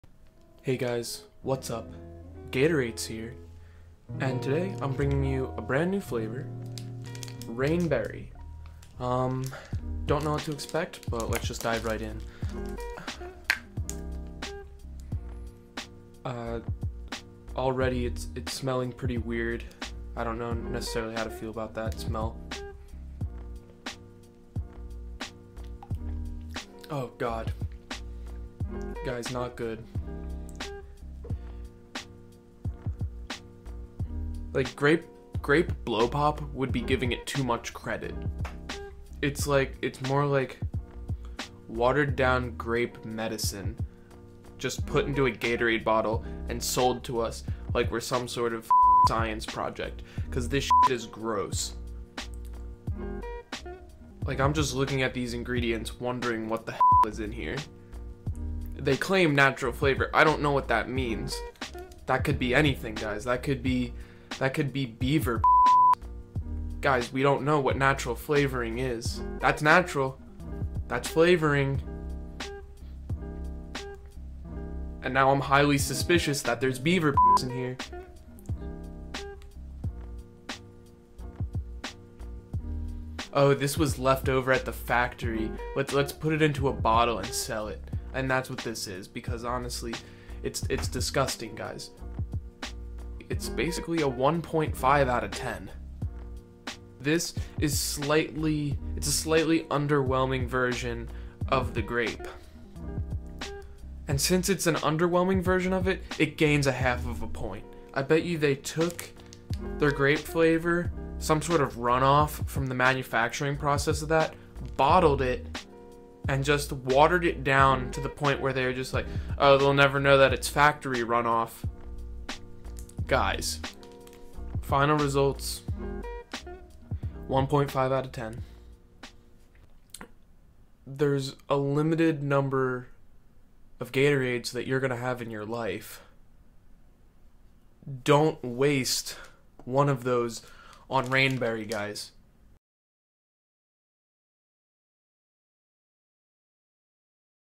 Gatorade FROST flavor Rain Berry: A Complete Disappointment
Gatorade FROST flavor ""Rain" "Berry"" gets a 1.5/10 score on the gatorates chart. This flavor is disgusting and reminds me of the Grape flavor of Gatorade FIERCE. There is no sourness, there is no sweetness (appropriate grape flavors). There is only a hint of artifical plastic and beaver piss. Sub 1-dimensional (less than 1 flavor is detected in the flavor profile) and a complete disappointment.